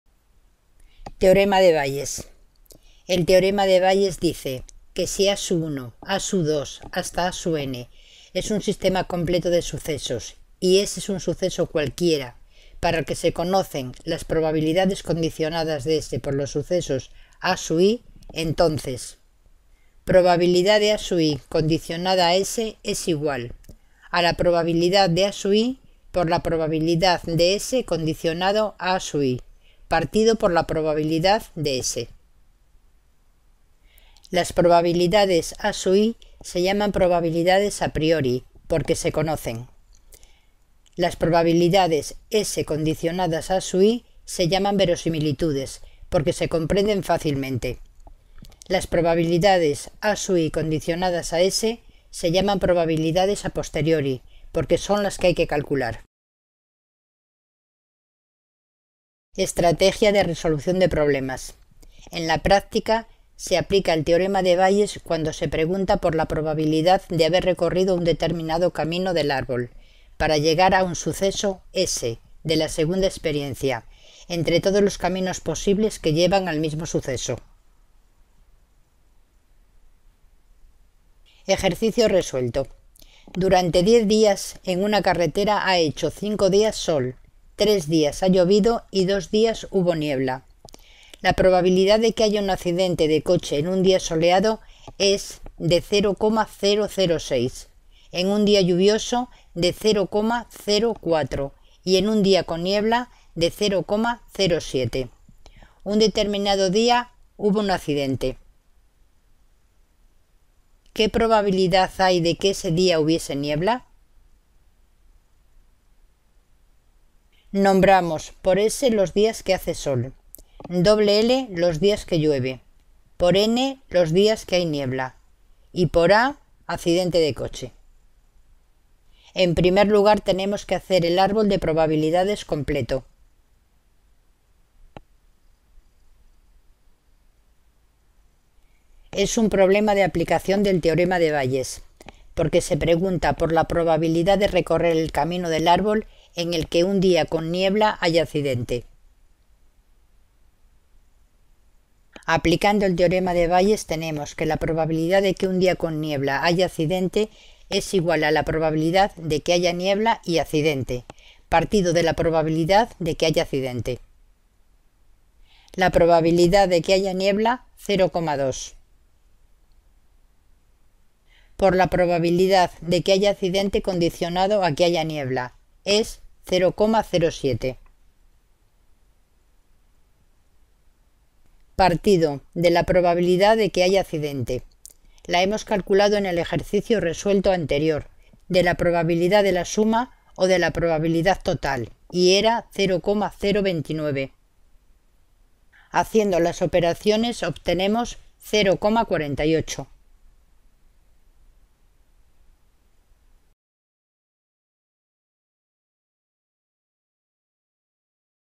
Probabilidad.Teorema de Bayes
Probabilidad.Teorema de Bayes: Presenta la teoría de forma muy clara y desarrolla paso a paso el mejor problema de este tipo.